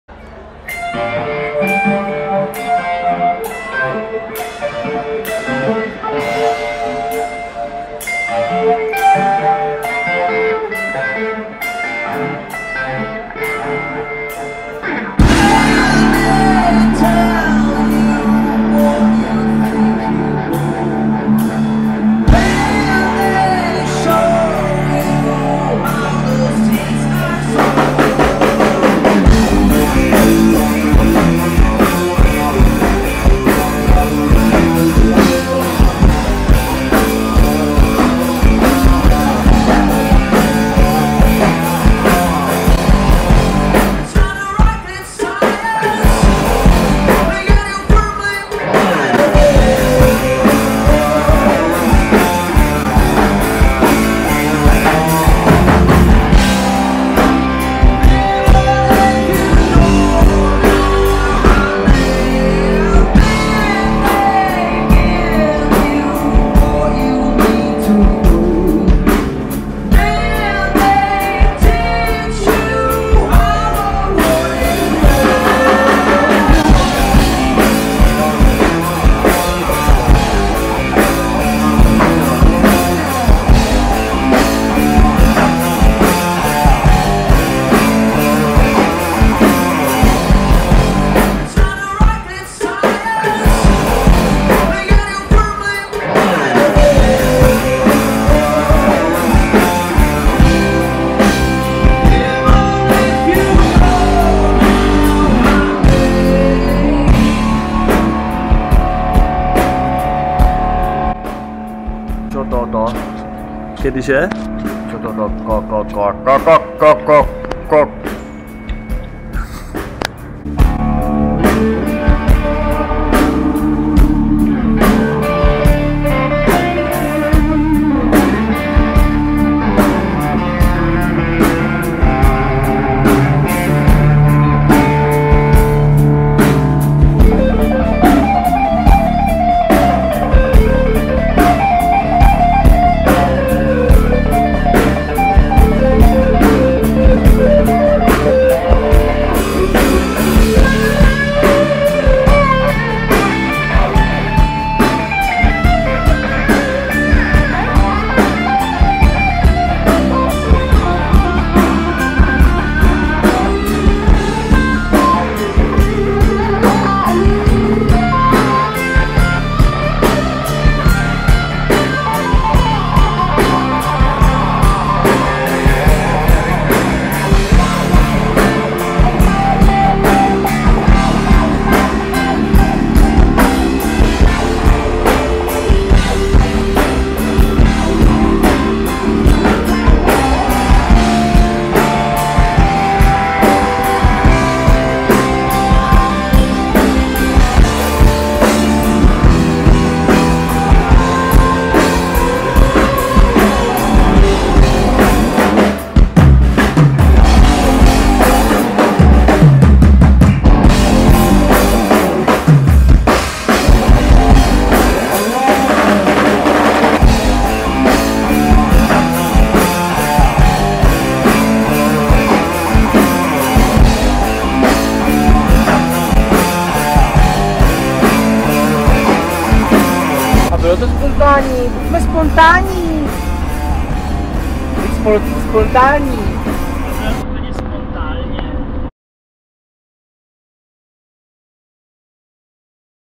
Aivn's Naked Trio in Bruxelles, 12.6.2015
A short footage from our gig in Belgium. The song being played is  one of the fresh tunes that we're preparing for the new album.

Camera: Aleš Alf Skoupý